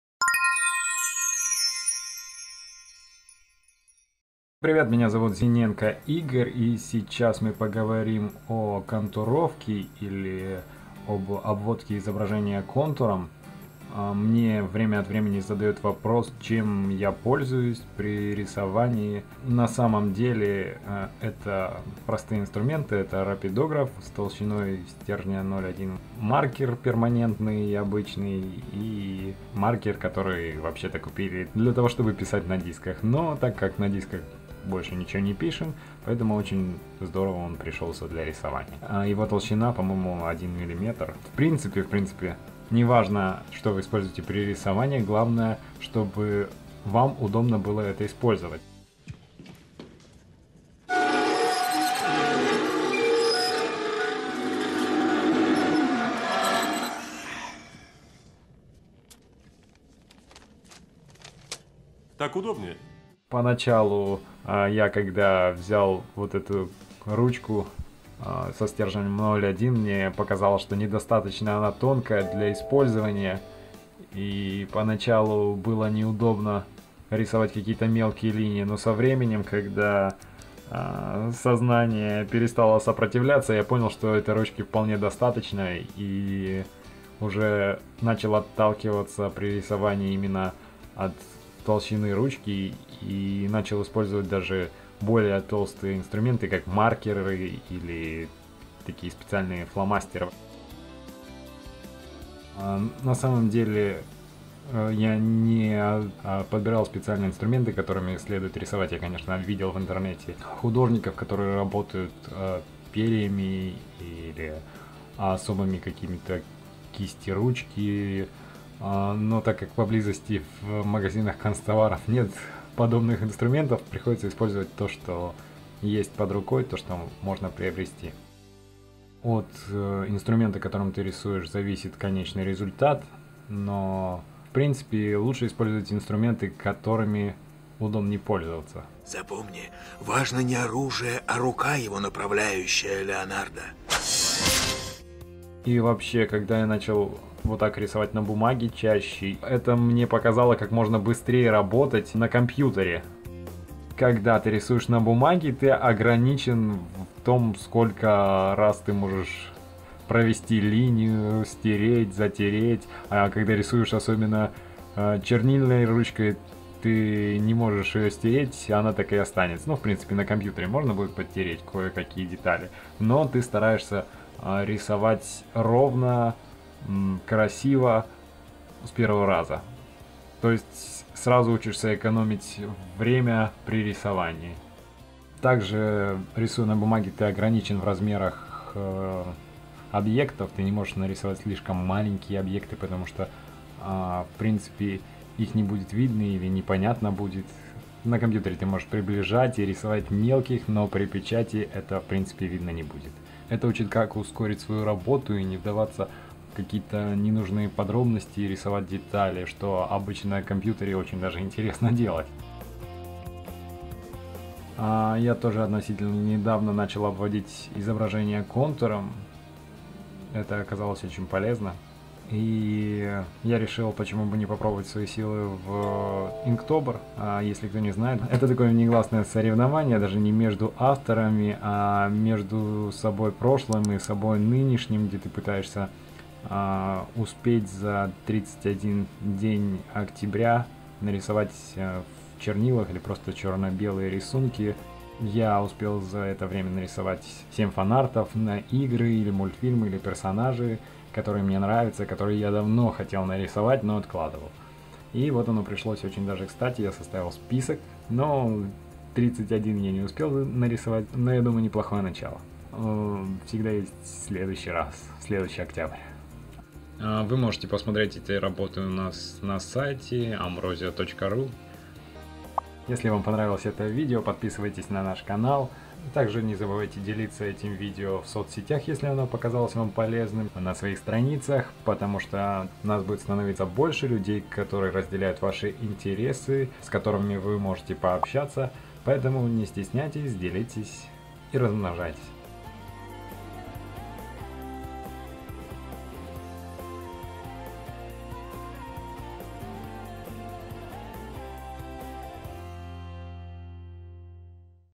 Inktober - Как обвести персонажа. Как рисовать лайн (скетчинг) - Навыки Рисонавта №9
🎨 В творчестве нельзя быть на 100% уверенным в конечном итоге. Даже после того, как ты тысячи раз рисовал одного и того же персонажа, ты всегда узнаёшь о нём что-то новое. Потому что в процессе ты не перестаёшь думать и чувствовать, представлять себя в роли этого героя. И это, пожалуй, самое классное.

ИЗ ЭТОГО ВЫПУСКА ВЫ УЗНАЕТЕ:
00:00 | Вступление
00:14 | Чем рисовать скетчи
01:02 | Практика делает мастером
02:25 | Зачем рисовать на бумаге, а не сразу на компе
03:35 | Что такое Inktober.